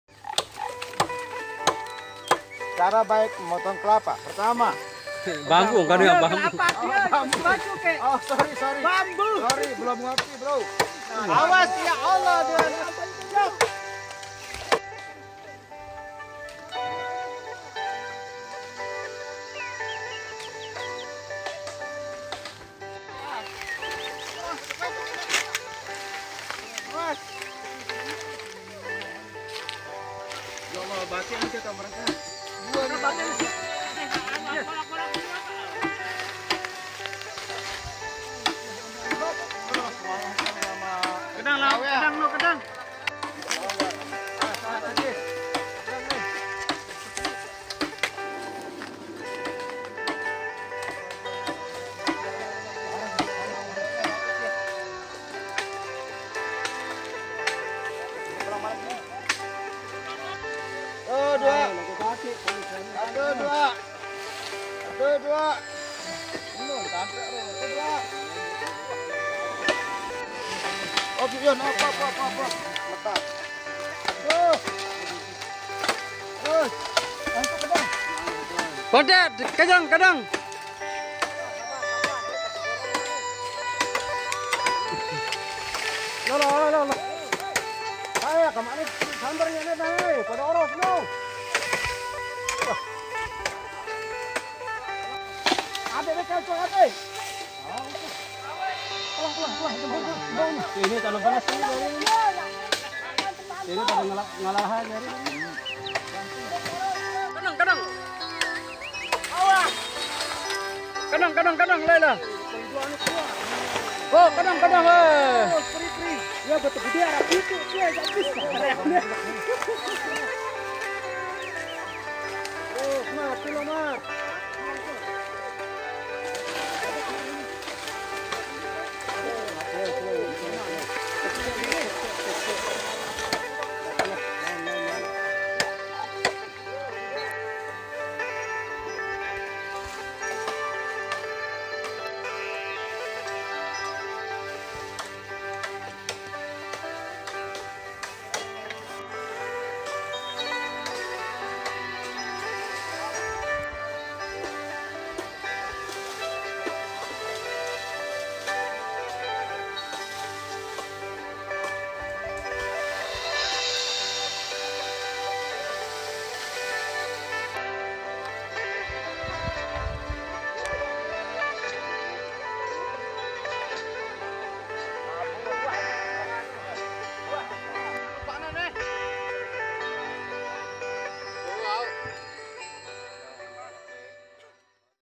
Tebang bambu di hutan angker sekotong timur lombok
Gotong royong Warga dasan ketujur untuk pembangunan masjid nurul hidayah